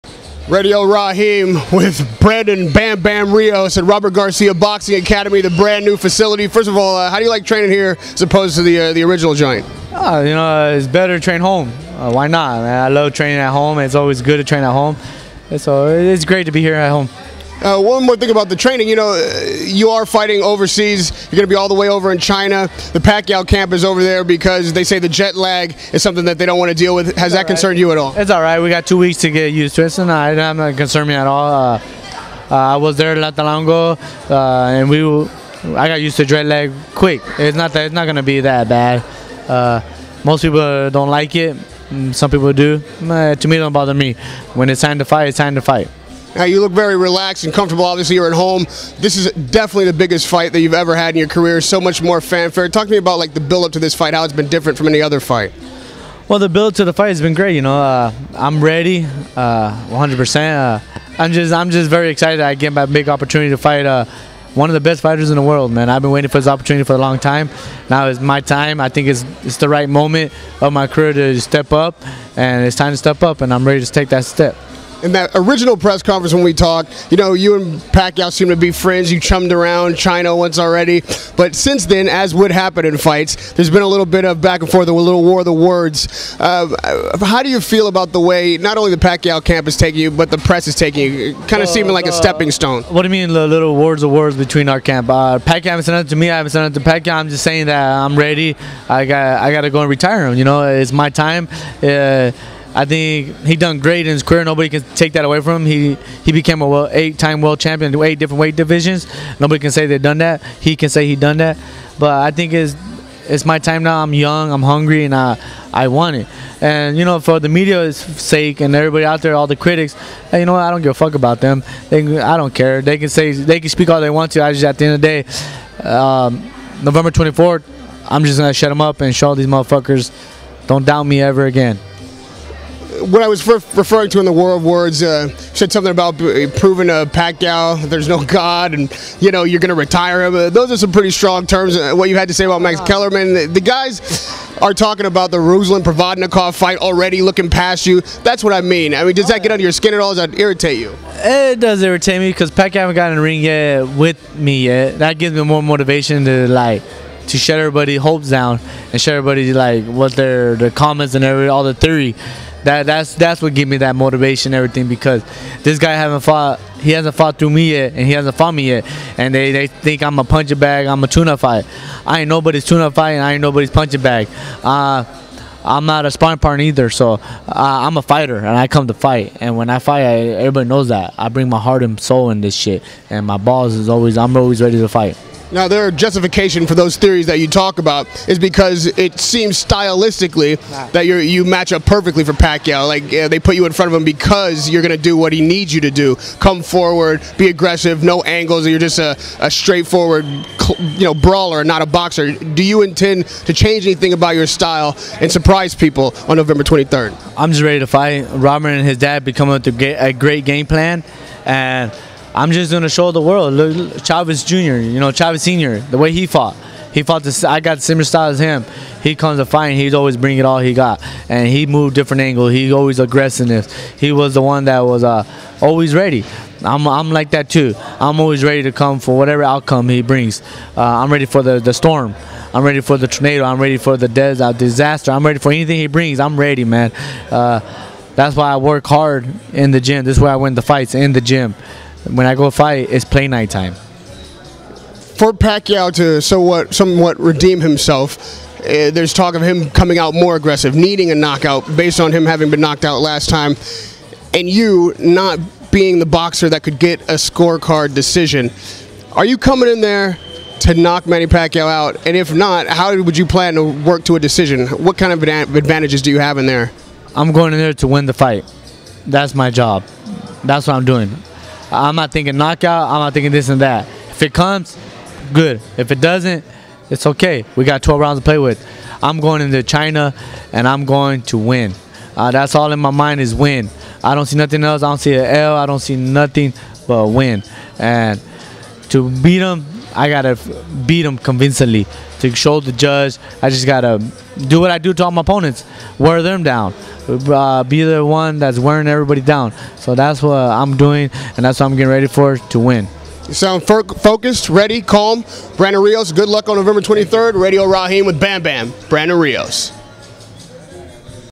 Brandon Rios "I Ain't Nobody's Punching Bag!" vs Manny Pacquiao
Radio Rahim Interviews Brandon Rios about his upcoming fight vs Manny Pacquiao. Brandon Rios media workout where Brandon Rios address disrespect from the media and Manny Pacquiao camp.